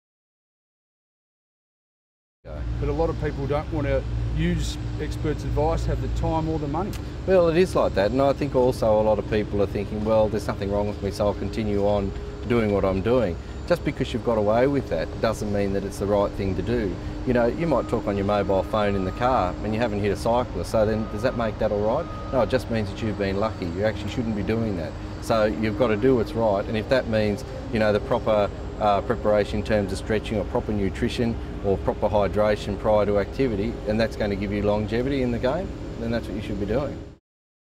LiveSafeTV Dr John Quinn - Are You an Expert.mov
Exercise is straightforward, right? You know what you're doing and what works for you. How much professional input should you get for your training programs?

LiveSafeTV speaks with Dr John Quinn, Exercise Physiologist and Elite Performance Coach at the Olympic Park Sports Medicine Centre and previously High Performance Coach with the Essendon Football Club (AFL).